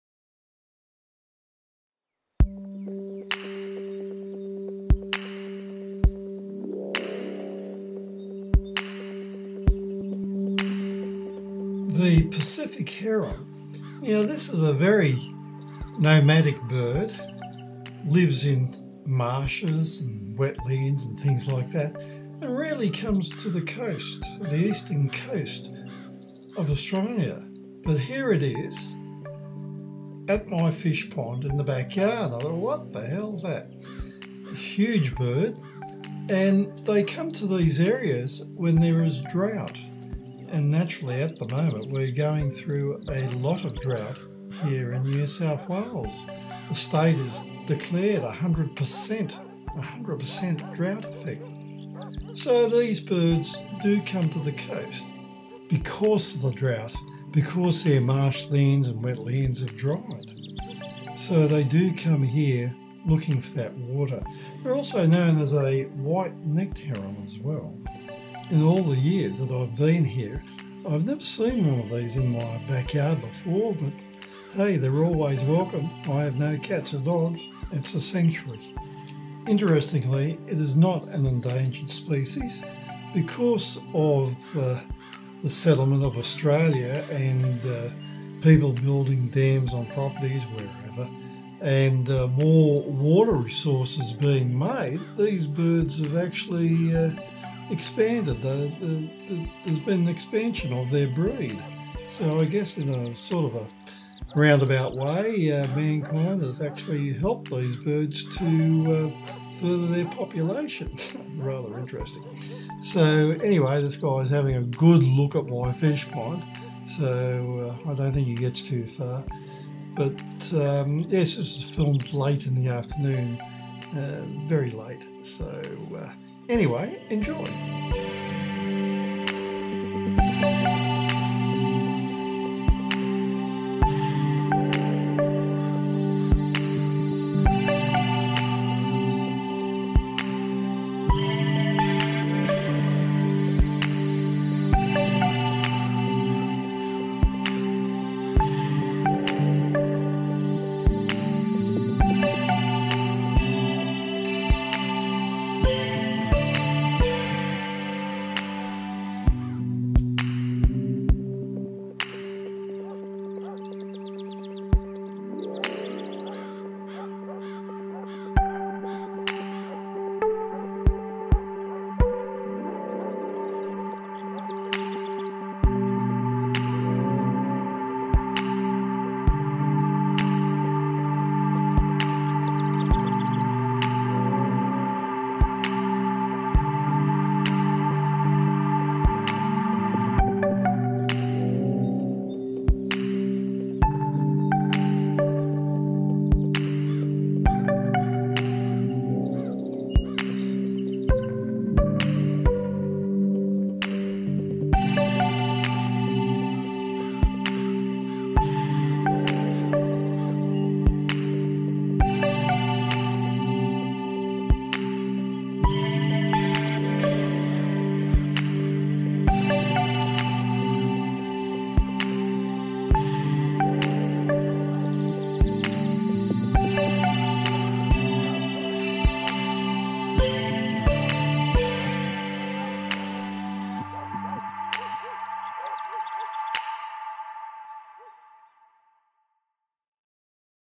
NATIVE BIRDS OF AUSTRALIA - PACIFIC HERON
BEST ON FULL SCREEN AT 720p.

This video is about a Pacific Heron that turned up in my backyard to sus out one of my fishponds.

These birds come to the Australian coast in times of drought as they are wetland birds. Currently New South Wales is in a 100% drought.

I've been here years and have never seen one before in my backyard which is like a sanctuary ( I have no cats or dogs..).